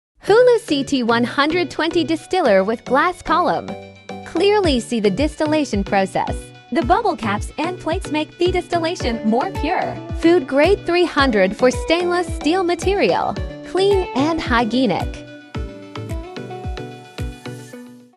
Water Bath Jacketed Distiller with Copper Helmet | HOOLOO CT120 Distillery Equipment
In this era where a single distillation unveils the pinnacle of brewing and excellence, the CT120 Distiller reinvigorates the art of brewing with its outstanding performance. Whether it's whiskey, vodka, or rum, a single distillation brings forth rich flavors, aromatic notes, truly epitomizing the essence of quality craftsmanship.

The Allure of Pure Copper, The Beauty of Details 🌈

Crafted with 99.95% pure copper, the core components of this distinctive distiller pay meticulous attention to every detail, ensuring a seamless fusion in every step of the brewing process. With water bath electric heating technology, temperature uniformity breaks the traditional brewing constraints, providing an unprecedented brewing experience.

Capacious Design, Unveiling Essence 🍇

The remarkable capacity of the CT120 Distiller leaves one in awe - a 120L double-layer jacket pot releases the essence of ingredients with each distillation. Coupled with dual operation windows and a 4" copper helmet, the liquid undergoes an exquisite sublimation process, resulting in a product of utmost purity and nobility.

Versatile Applications, Brewing Without Bounds 🌐

With extensive applications, the CT120 Distiller is a jack-of-all-trades. Whiskey, vodka, fruity spirits, brandy, gin, rum... With just one machine, it effortlessly caters to diverse alcoholic beverage needs, letting you master various flavors with ease.

Fashionable Exterior, Artistic Beauty 🎨

The design of the CT120 Distiller is enchanting. The copper sheen glistens under the light, as if born for fine spirits. The stylish lines create a canvas of craftsmanship, making this machine not only a practical tool but also a vibrant highlight in the kitchen.

A Decade of Refinement, Brewing a Colorful Life 🌈

Drawing on a decade of industry expertise, the CT120 Distiller stands as an indispensable ally in the eyes of master brewers. Let's embark on a journey to explore the coveted world of brewing, where each drop of liquor becomes a profound imprint of time and passion. CT120 Distiller - savor life, brew brilliance! 🍷✨

Follow us on other Social Medias and AliExpress to get the latest news!
On AliExpress